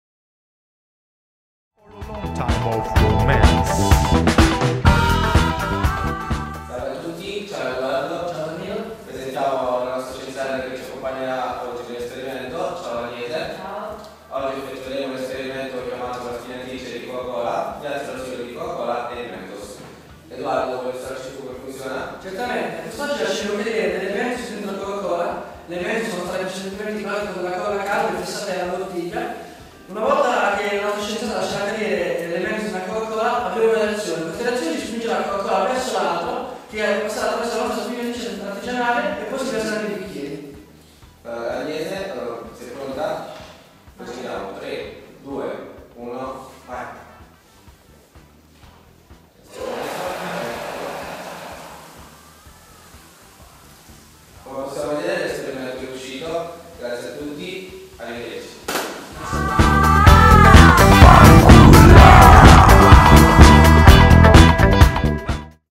Esperimenti al Corinaldesi: dispenser coca e mentos
Esperimenti svolti dai ragazzi dell'Istituto Corinaldesi di Senigallia durante dei laboratori organizzati da fosforo.